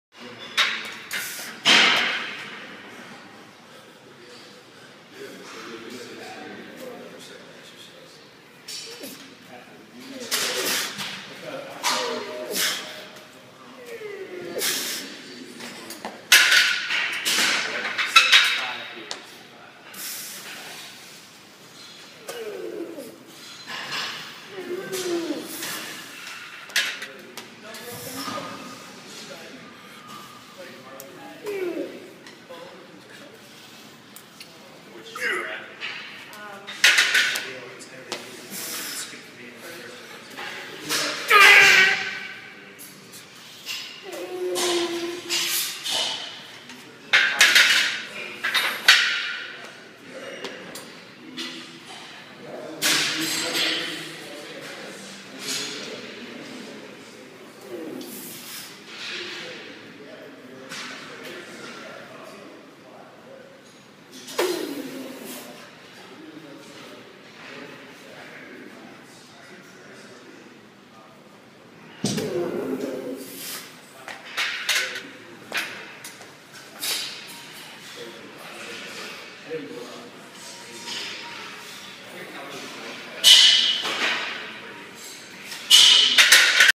Speed Squat 10/7/13
ByrdCAT Squats
5x5x325, one minute rest

Accommodating Resistance Squats
3x2x315+ orange bands
3x2x315+ grey bands

Front Squats
3x3x225, four-count pause
275,335x1

Barbell Hip Thrusts
405x10
495x7
315x20

Giant Set: Leg Extensions/Leg Curls/Band Pull-Aparts/Band Face Pulls
3x20x200/120/red band/orange band